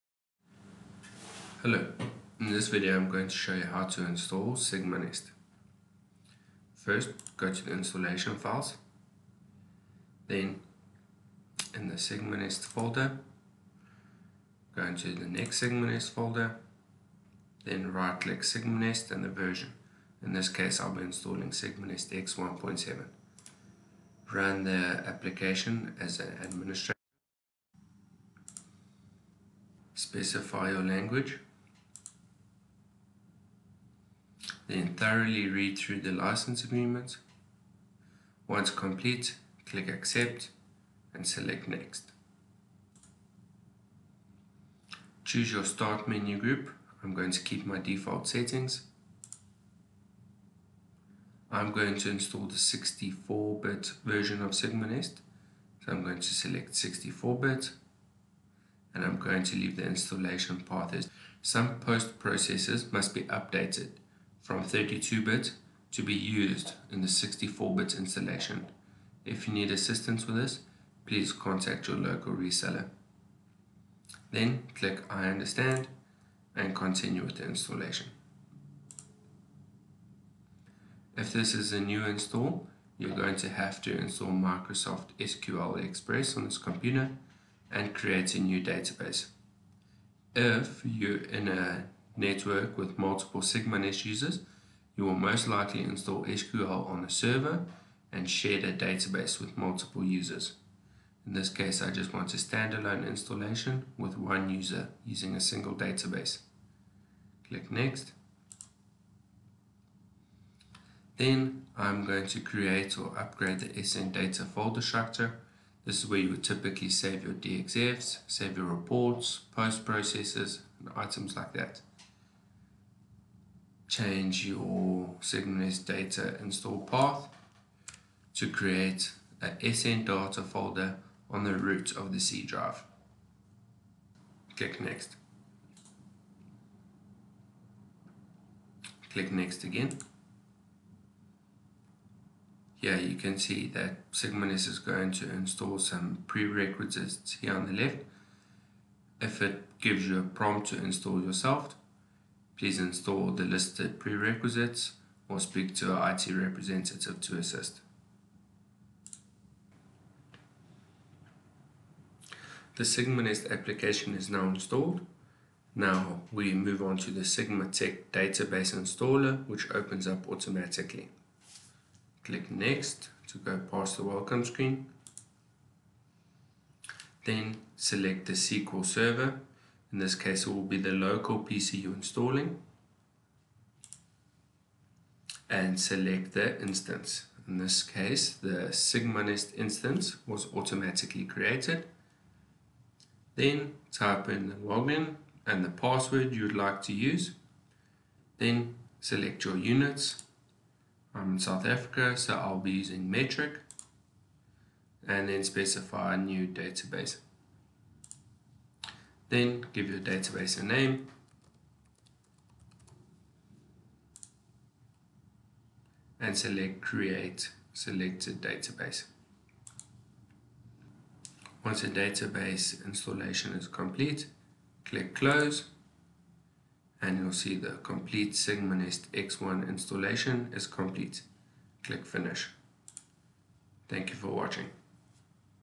SigmaNEST installation
SOLIDWORKS & MECAD bring you this step-by-step guide on How to Install SigmaNEST quickly and correctly. In this video, we walk you through every stage of the installation process to ensure smooth setup and optimal performance. Perfect for new users or anyone needing a clear refresher.

🔧 What You’ll Learn (Timestamps)

00:00 – Introduction

00:10 – Installation files

02:40 – Once SigmaNEST is installed